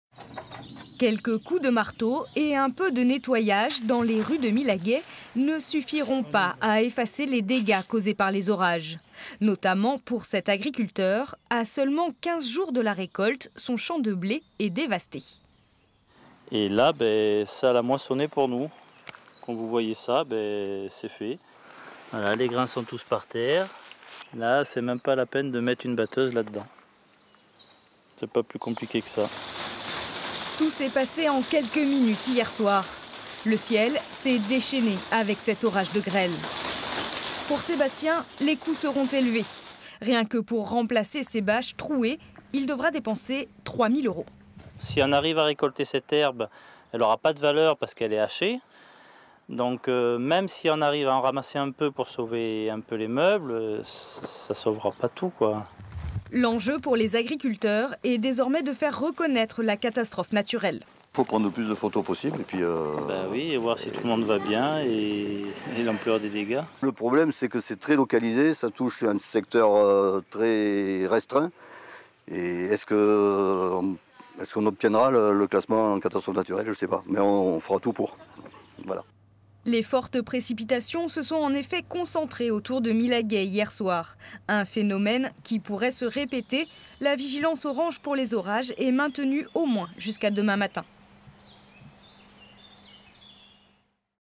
orages milhaguet marval haute-vienne
Dans la nuit du 9 au 10 juin 2018 de violents orages ont frappé la commune de Marval en Haute-Vienne.
Lieu : Marval (haute-Vienne)
Intervenants : Sébastien Léger (agriculteur) ; alain Perche (maire de Marval)
Reportage : Pauline Pidoux, André Abalo, antonio Dos Santos